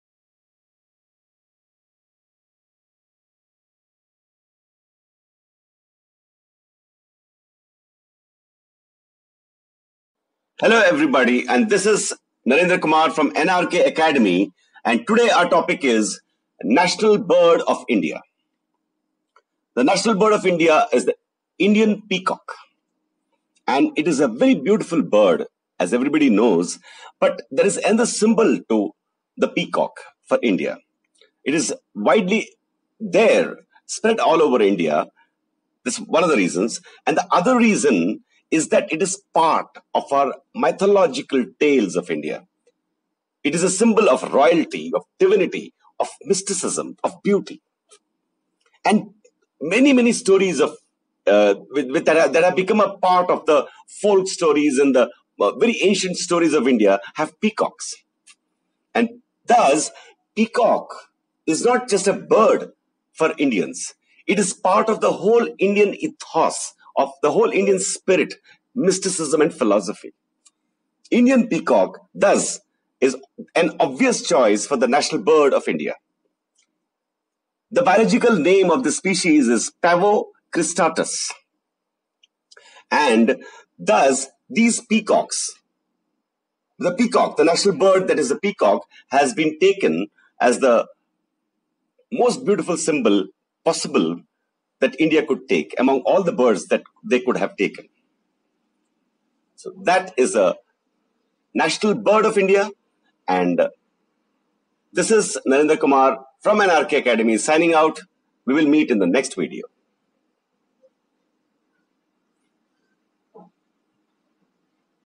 NATIONAL BIRD OF INDIA
This is a video that beautifully expresses the meaning and significance of the peacock as the national bird of India.